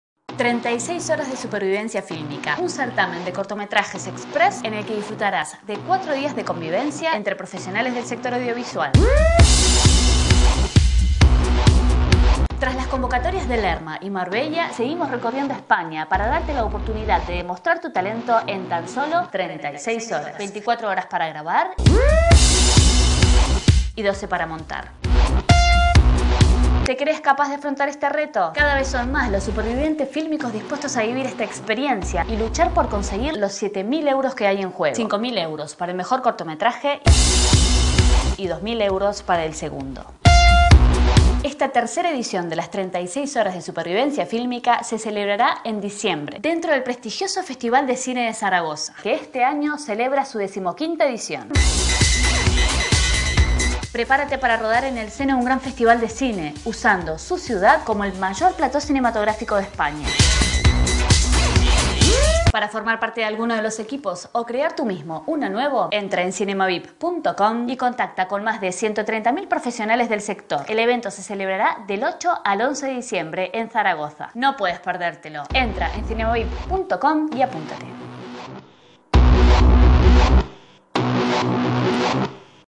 Promo 36 horas de supervivencia fílmica en Zaragoza
Certamen de supervivencia filmica se desarrollo en FCZ 2010